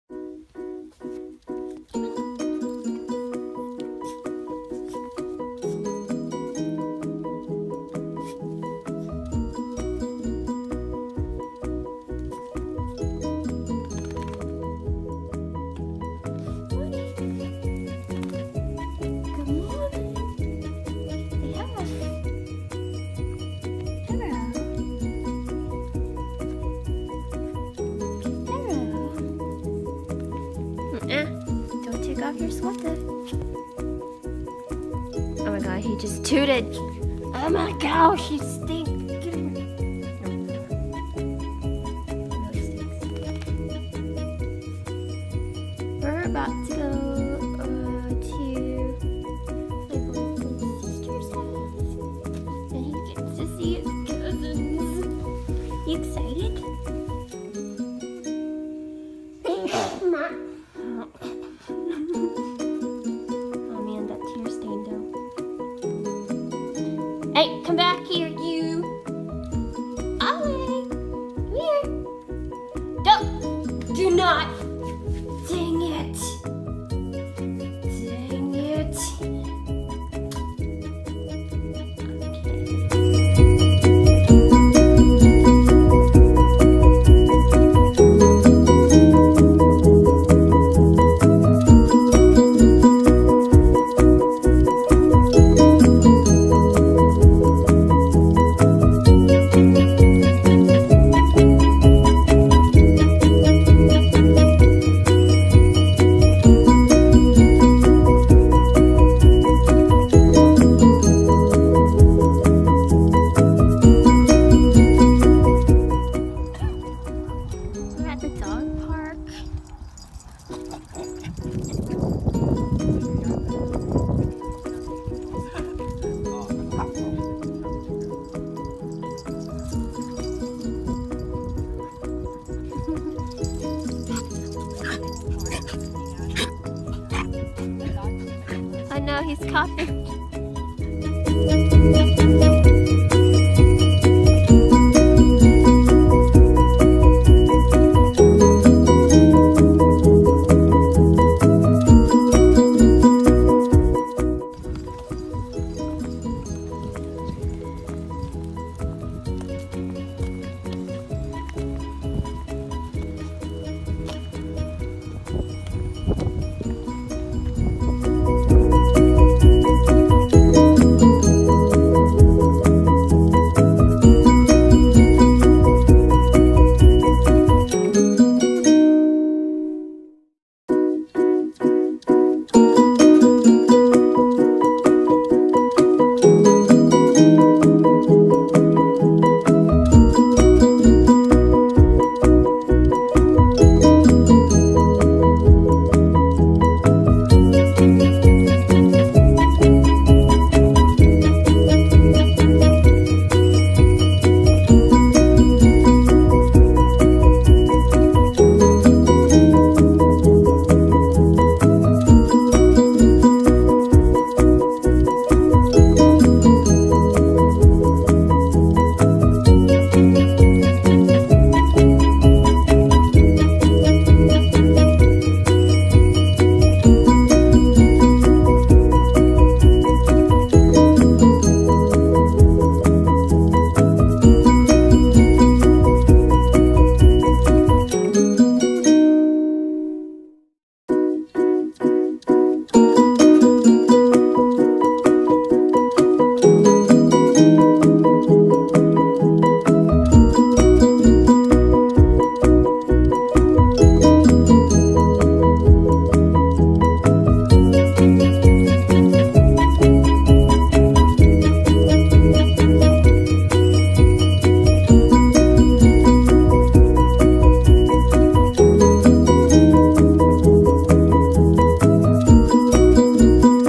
First Time At Dog Park!
Ollie's first time at a legit dog park! He had a blast meeting so many new friends.
Lucky and Brownie did as well. The boys were really well behaved and played nicely with everyone.
Sorry for the sort footage, I was too busy watching over Ollie to make sure he wasn't causing trouble lol.
I'll try my best to film more next time.
Also, follow Ollie on instagram at Olliethepartiyorkie